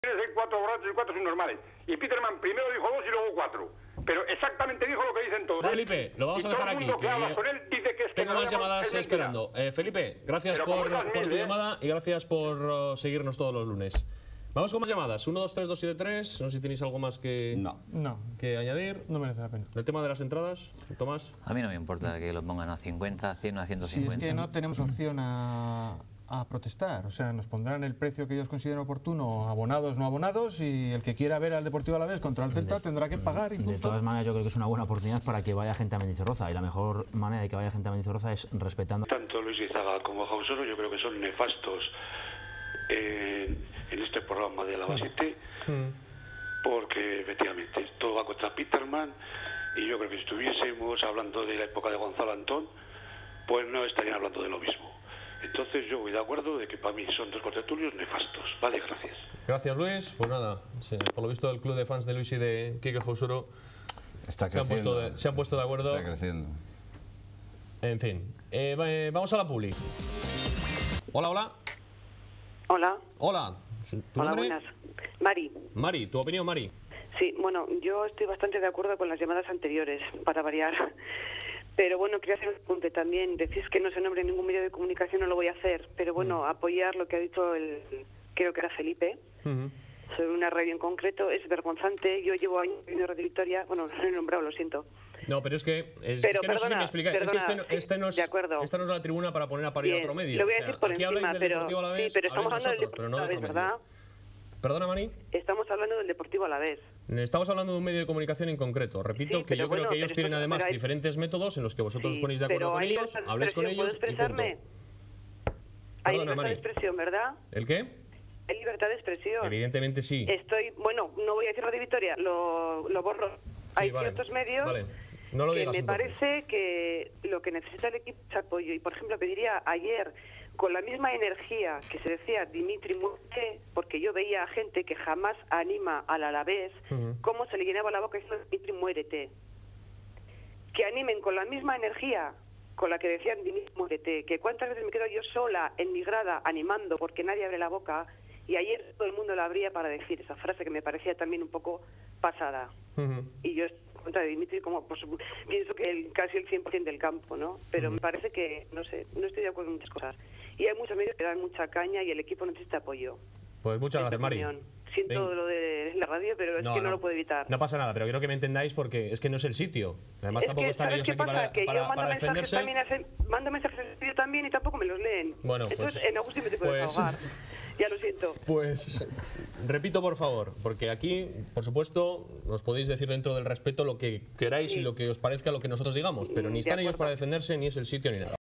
Lio en Alava 7
Criticas a Izaga y a Jausoro contertulios del programa local Mas que un juego de Alava 7 Televisión.